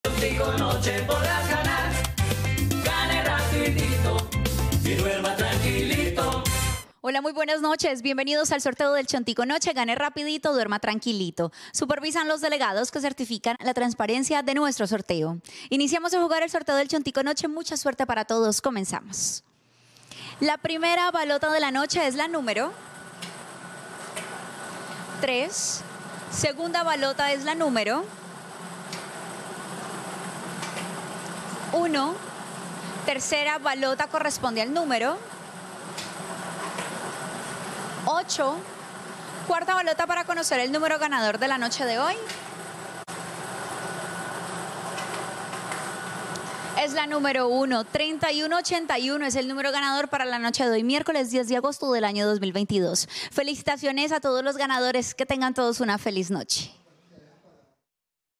CHONTICO NOCHE Resultado del MIÉRCOLES 10 DE AGOSTO de 2022 ✅🥇🔥💰
Hola buenas noches, te presentamos los resultados de las loterías y chances en Colombia, vamos con el Resultados del Chance El Chontico noche del MIÉRCOLES 10 DE AGOSTO de 2022.